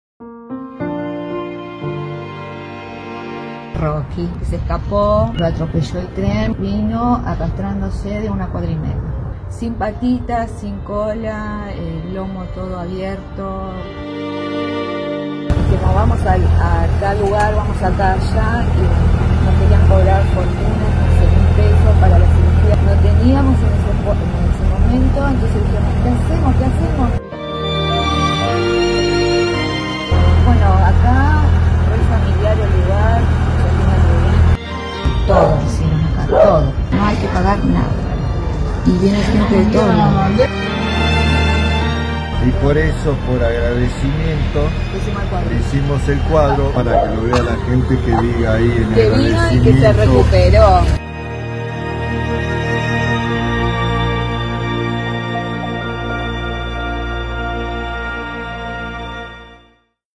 HOSPITAL VETERINARIO DE MALVINAS ARGENTINAS: QUÉ LE PASÓ A ROCKY
La familia de Rocky🐶 no sabía qué hacer hasta que llegaron al HospitalVeterinario de MalvinasArgentinas